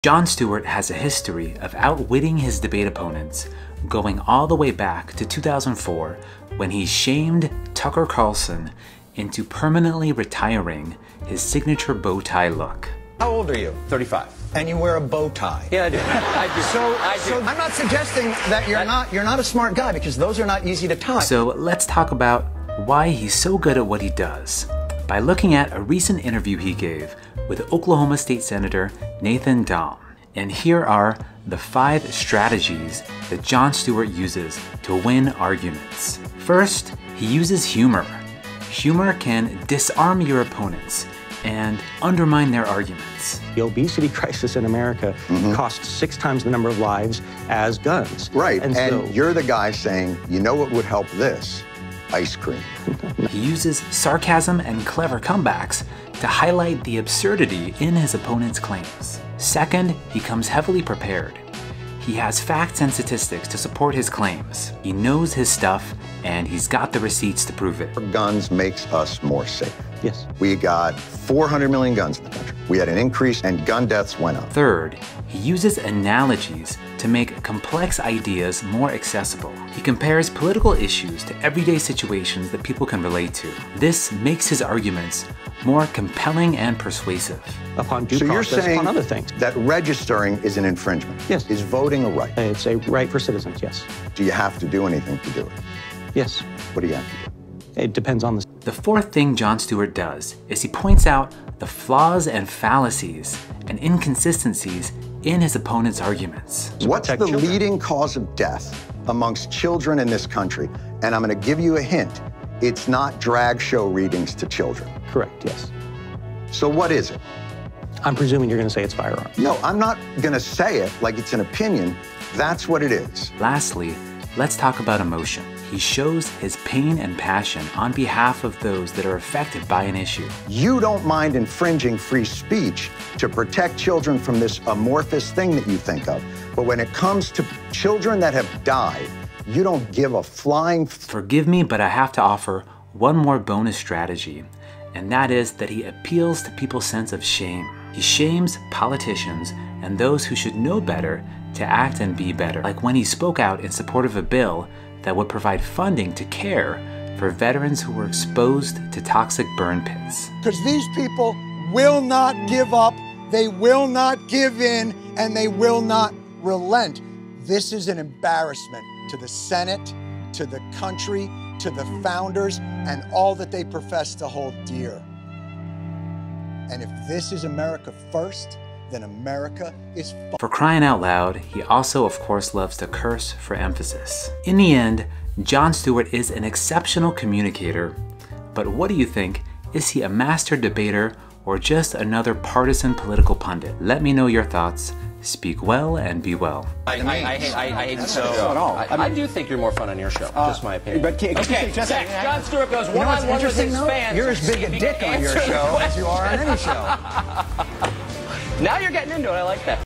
Jon Stewart's Top 5 Debate Strategies: How He Always Wins
Communication coach on Jon Stewart's debate skills. I look at his recent interview along with some other relevant clips. Let me know what you think.